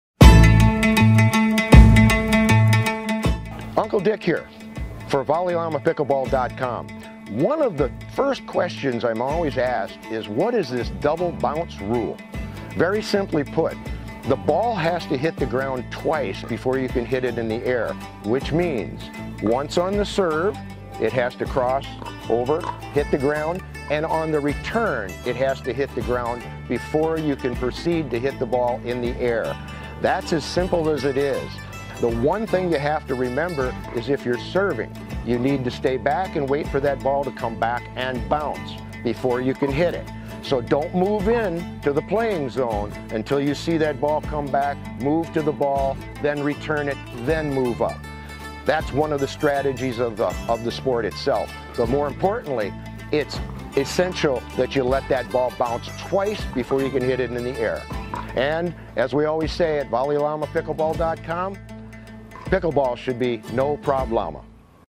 WHAT IS THE DOUBLE BOUNCE RULE IN PICKLEBALL | PICKLEBALL LESSON 3 | VOLLEYLLAMAPICKLEBALL
What is the double bounce rule in pickleball? It's actually pretty simple once you see it played in real life.  The double bounce rule is how you start the match.  The ball has to hit the ground twice before you can hit it out of the air.  This means once on the serve, then it crosses over the net where it bounces again and is then hit on the return for a second bounce.  At that point, you may begin hitting the ball out of the air as it crosses over the net without it touching the ground again.

VolleyLlamaPickleball.com - "Pickleball should be No-Prob-Llama!"